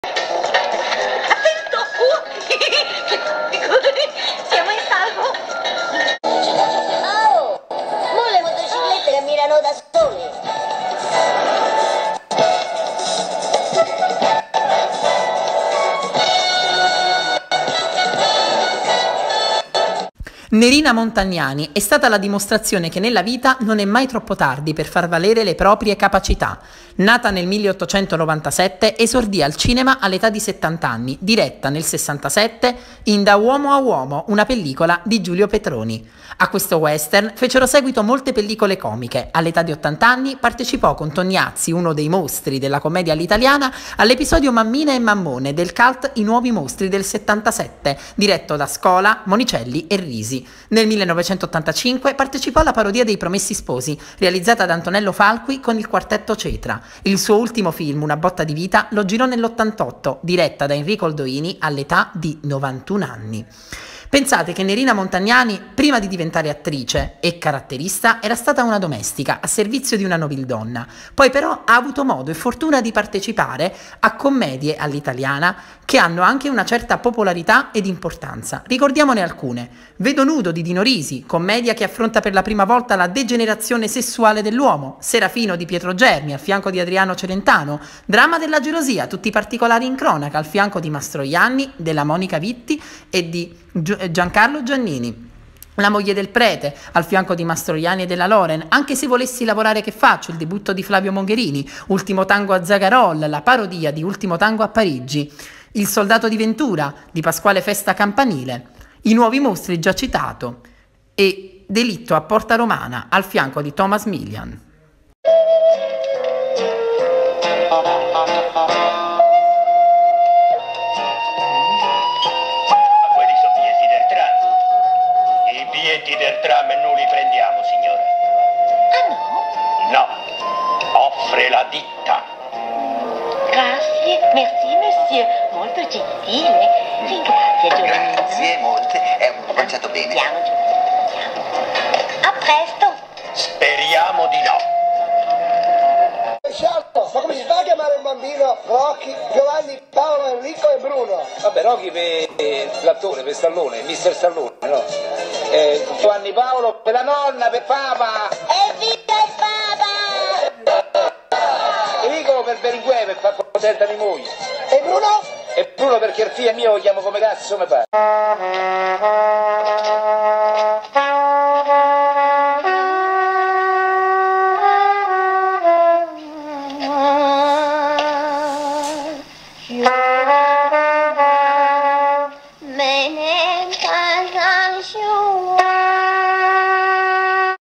La protagonista: Nerina Montagnani 🎬
Oggi celebriamo l’anniversario di nascita di un’attrice che debuttò al cinema a 70 anni, ma seppe farsi valere fino alla fine della sua vita come autentica e gradevole caratterista della Commedia all’Italiana, con partecipazioni di rilievo a commedie di spessore o popolari.

Una clip per scoprire il meglio della sua carriera! ❤️